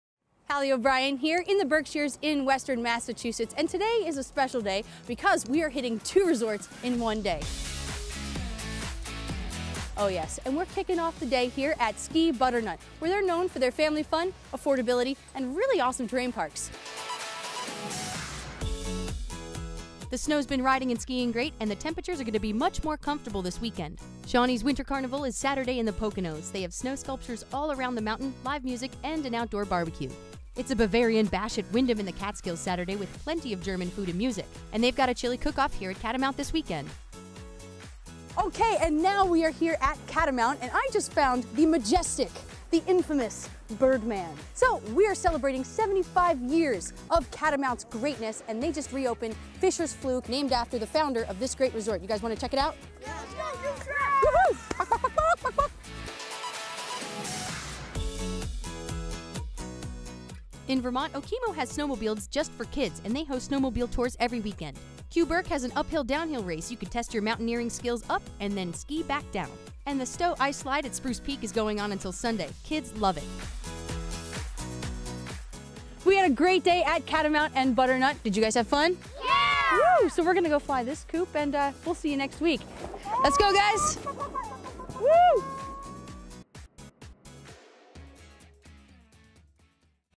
Ski and snowboard update: Halley O'Brien checks in from Ski Butternut and Catamount
Ski and snowboard update: Halley O&apos;Brien checks in from Ski Butternut and Catamount in the Berkshires of western Massachusetts about a two and a half hour drive north of Newark, N.J.